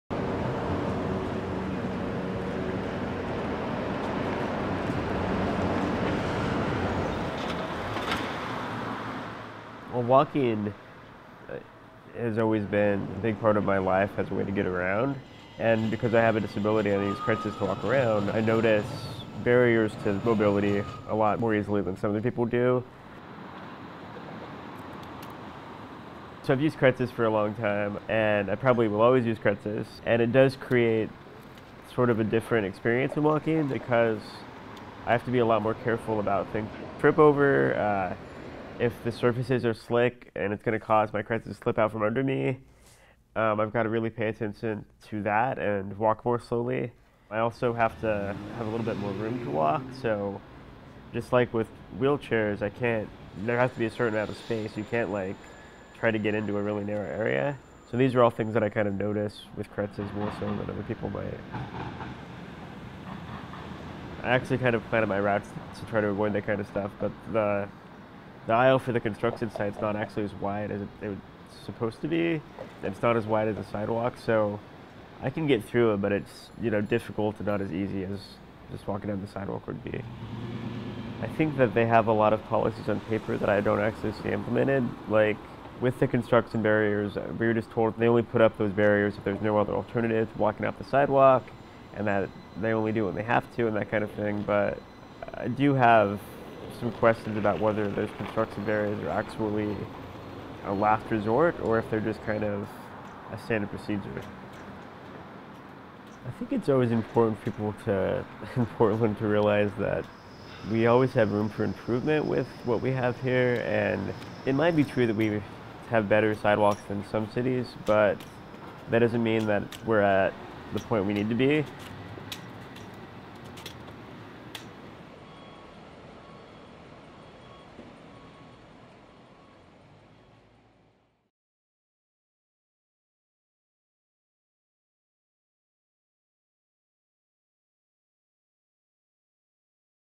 PedPDX: Walking With Matthew
Created in conjunction with the University of Oregon's School of Journalism PDX and PBOT's PedPDX Pedestrian Plan.

PedPDX: Portland's Citywide Pedestrian Plan will prioritize sidewalks, crossing improvements, and other investments to make walking safer and more comfortable across the city. The City of Portland Bureau of Transportation (PBOT) aims to elevate public understanding of the range and diversity of needs that Portlanders have when it comes to walking. Our video project supports these aims.

Learn more about PedPDX, the City of Portland's Pedestrian Master Plan,